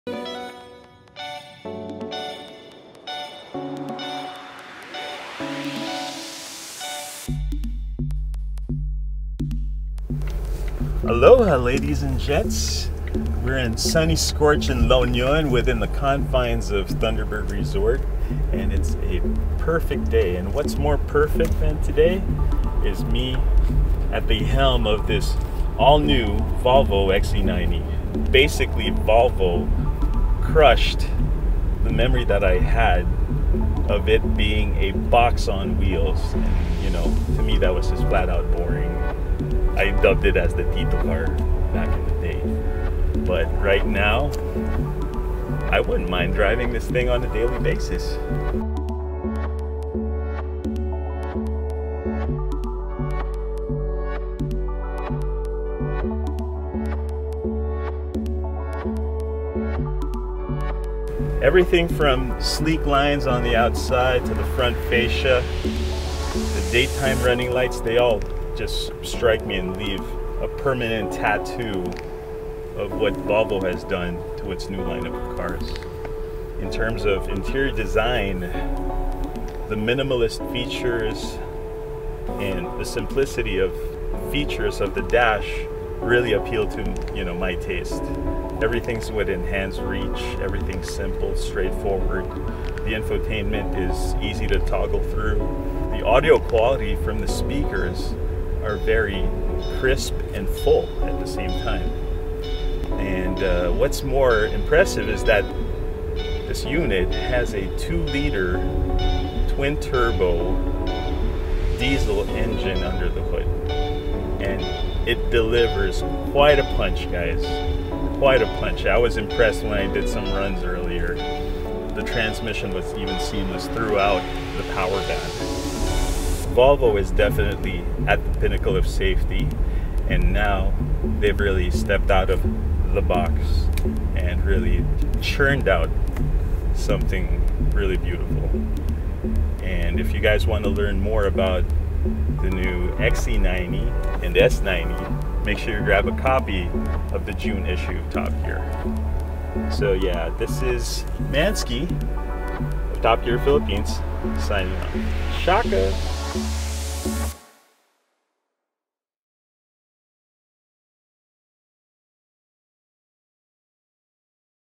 Volvo XC90: No longer a box on wheels
In this Top Gear drive, we drive up to the beautiful, scenic La Union to test drive the Volvo XC90. The Swedish wagon is packed to the brim with safety features, as well as many luxurious amenities worthy of its price tag. Click play to take a ride with us. It's a drive we won't soon forget.

Dig cars?